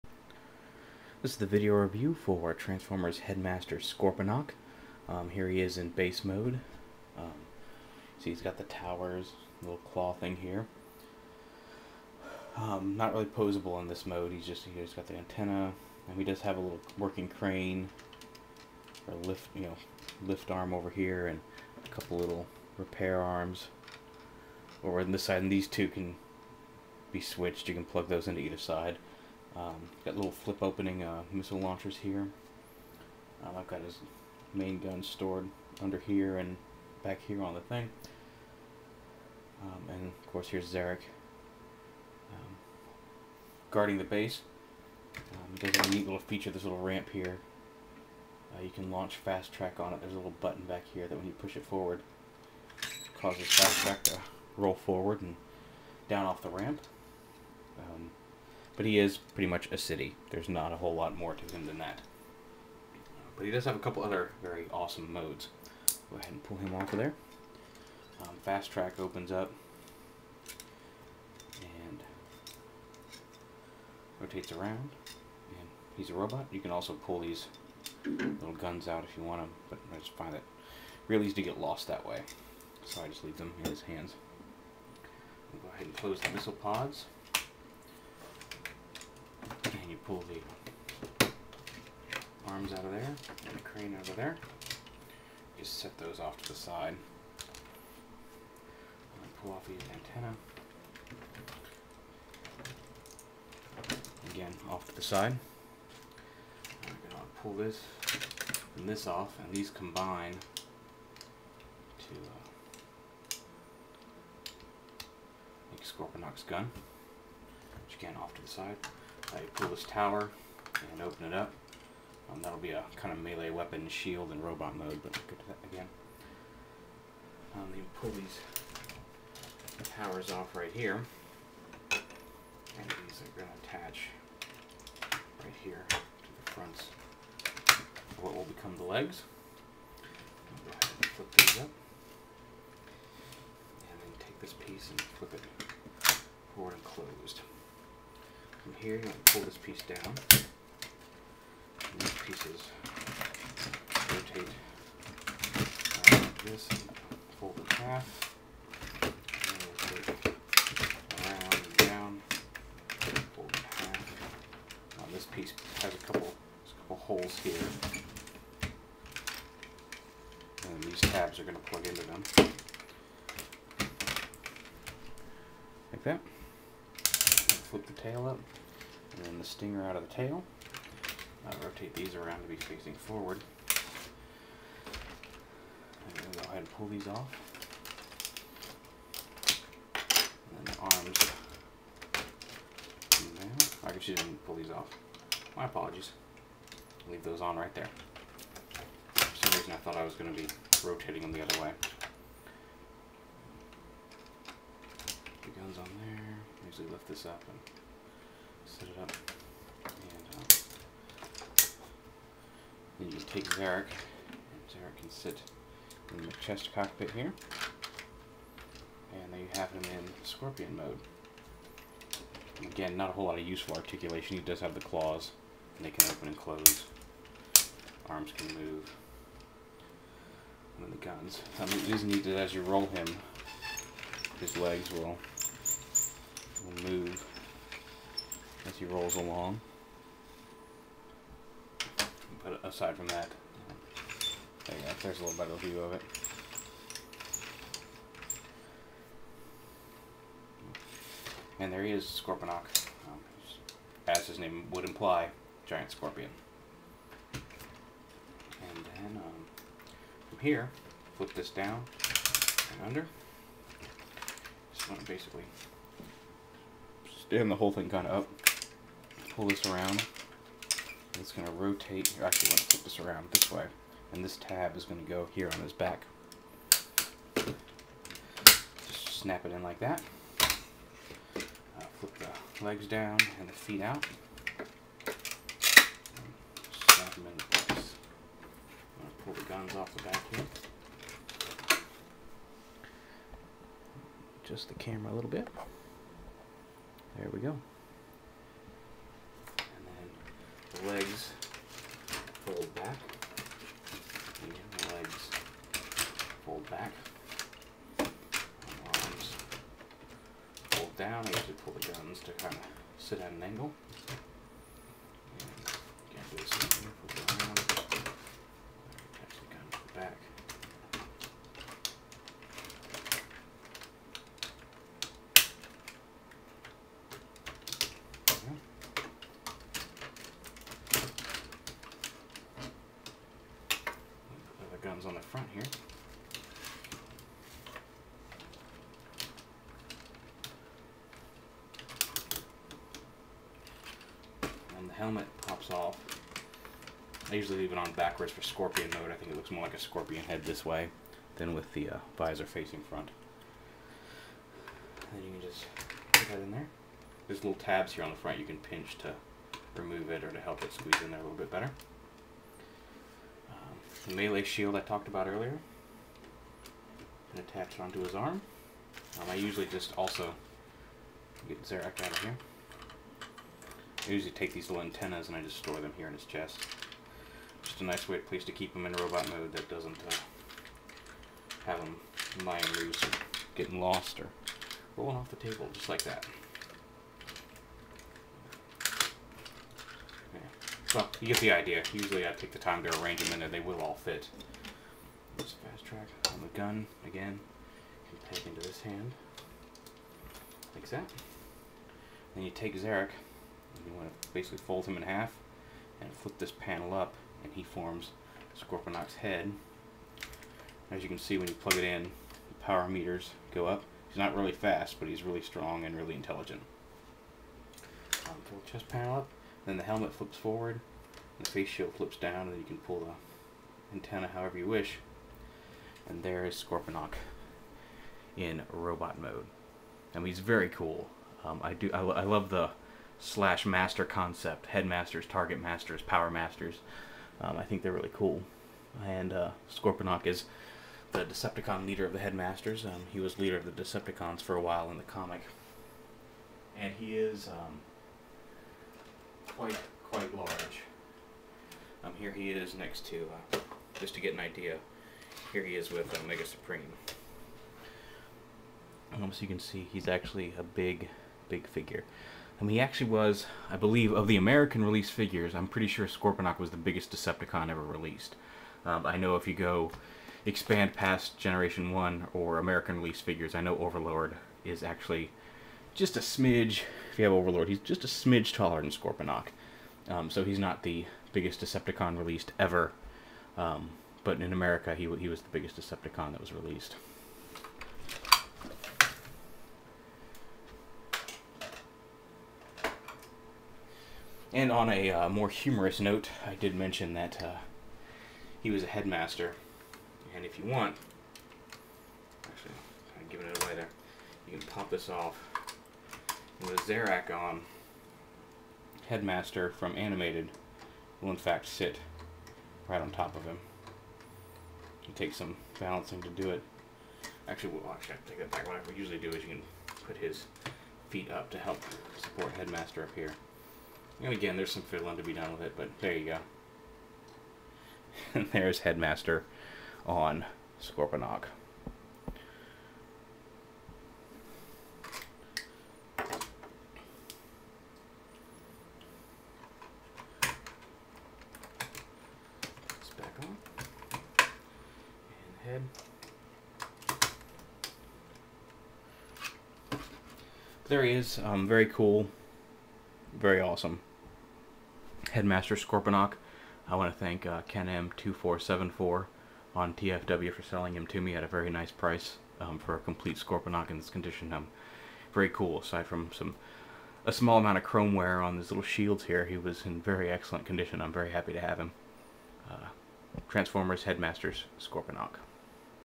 Transformers G1 Headmaster Scorponok
My review of Generation 1 Headmaster Scorponok.